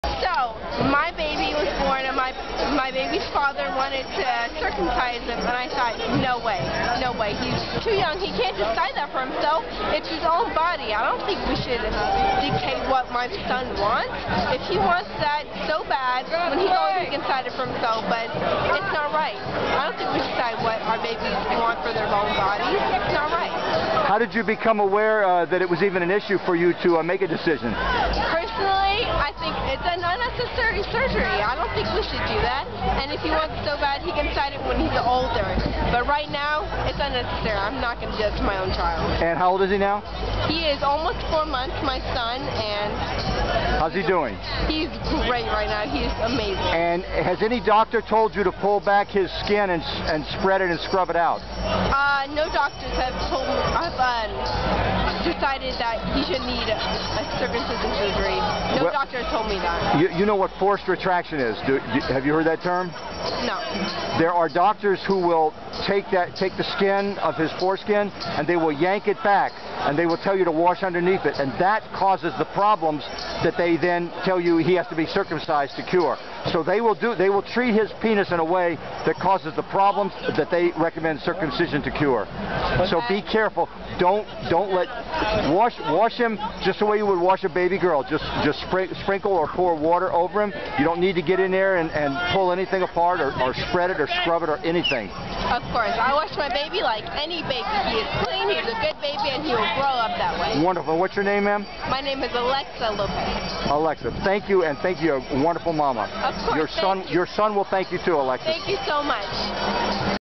NO WAY! My baby's father wanted to circumcise him.
"It's his own body. I don't think we should dictate what my son wants." ~Alexa, interviewed at SF Pride 2014